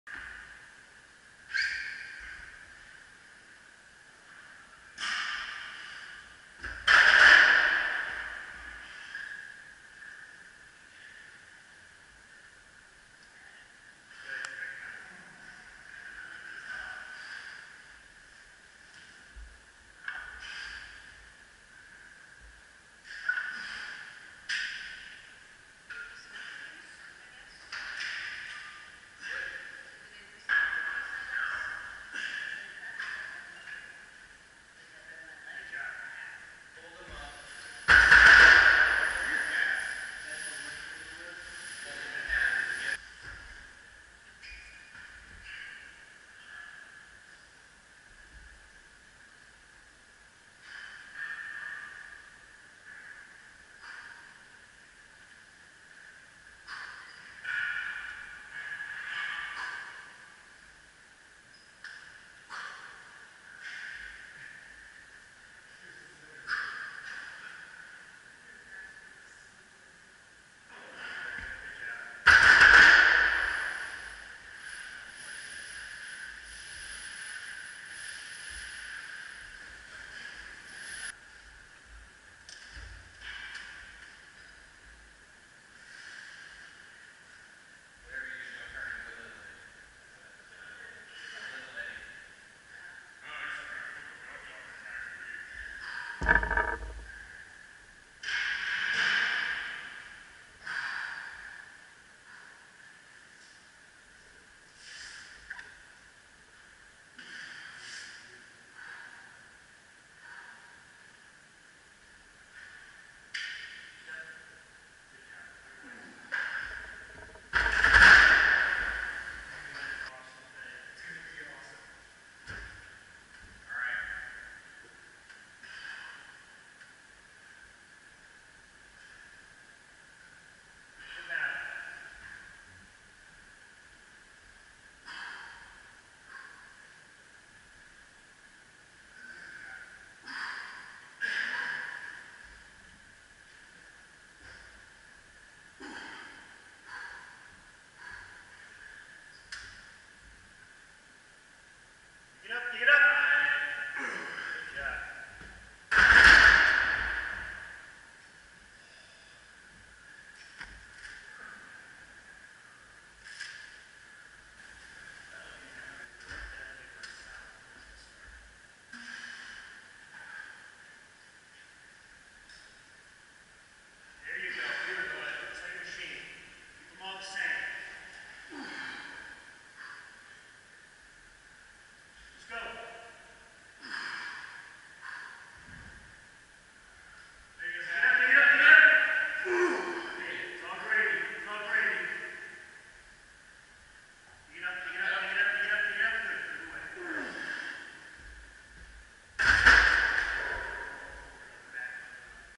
Squats: 425x5x3
Front Squats up to 270 lbs. for 8 and then 7 reps. Back Squats up to 425 lbs. for three sets of five.